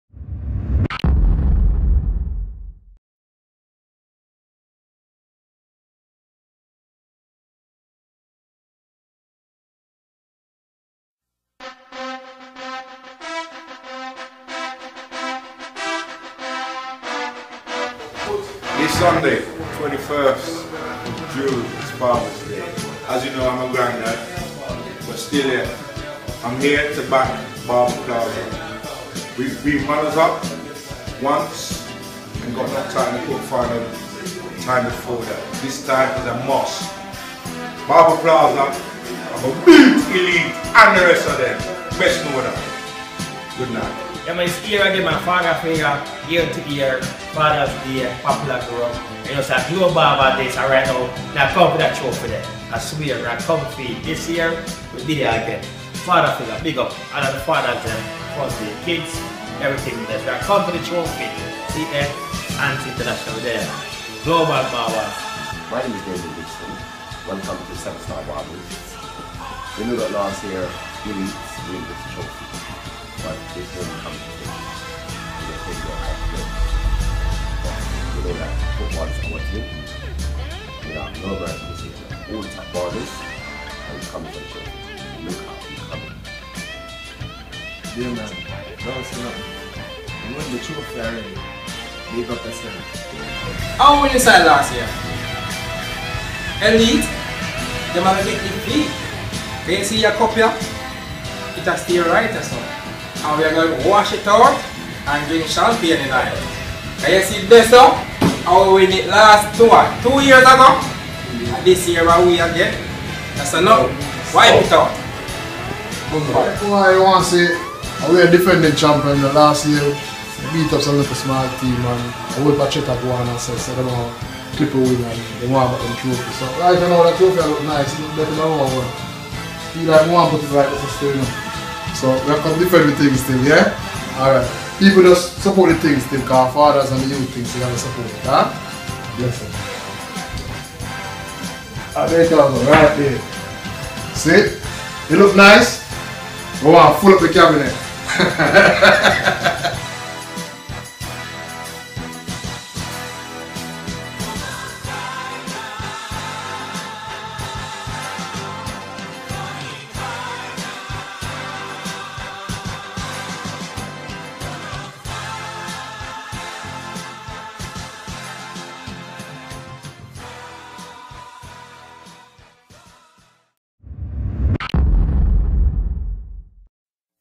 Barbers & Fathers Cup 2015 Trailer
Fathers Day 2015 will see the 3rd annual Barbers & Fathers 5 aside Cup! Elite barbers will be defending their title @ Poplar Grove Youth & Community Centre Poplar  Grove Wembley HA9 9DB